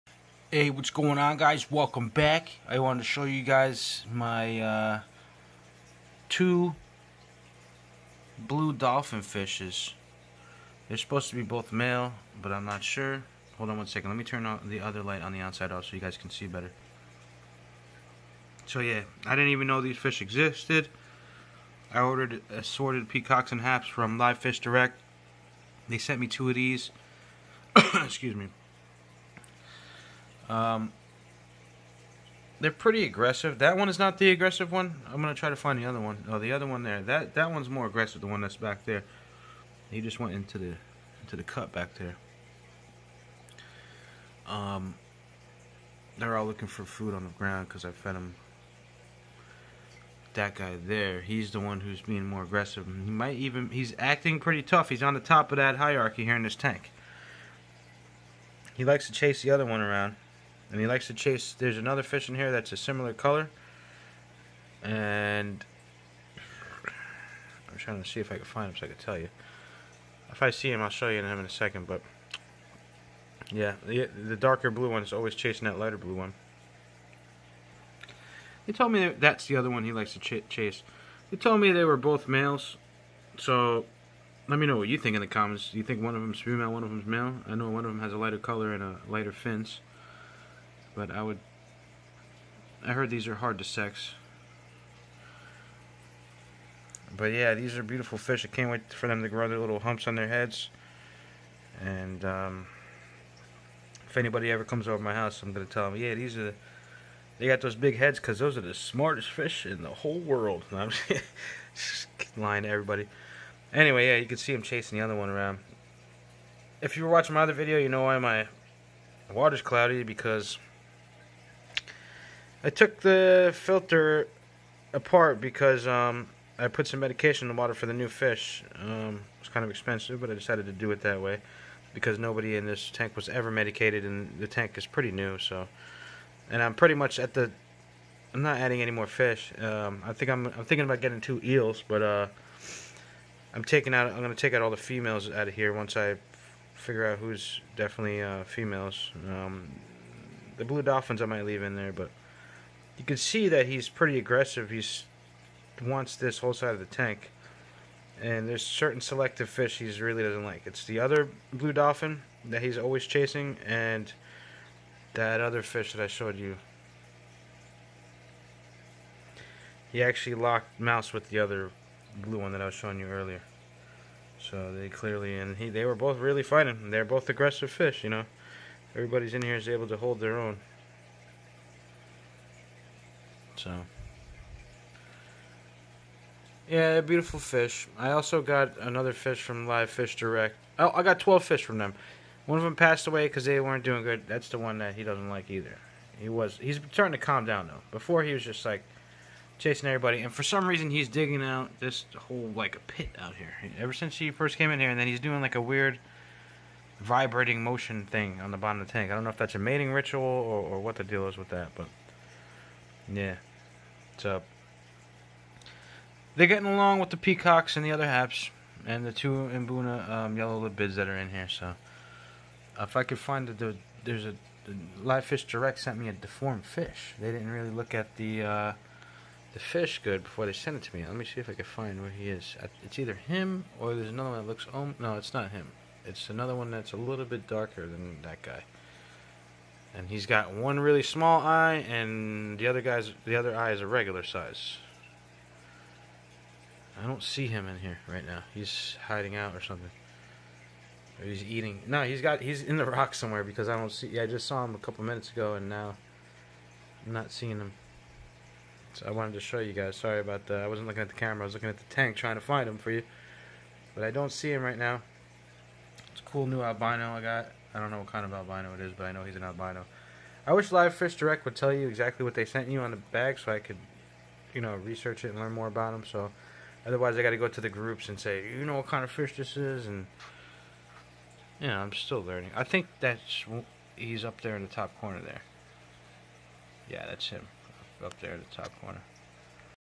2 blue dolphin african cichlid fish getting along with peacocks and other haps and 2 mbuna
Cyrtocara moorii, commonly known as the hump-head, is a species of haplochromine cichlid endemic to Lake Malawi in east Africa where they prefer areas with sandy substrates. It can grow to a length of 20 centimetres (7.9 in) TL. The species is popular among aquarium keepers where it is known as the hump-head cichlid, blue dolphin cichlid, Malawi dolphin or simply as moorii. It is currently the only known member of its genus.[2] The specific name honours the English cytologist and biologist John Edmund Sharrock Moore (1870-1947).[3]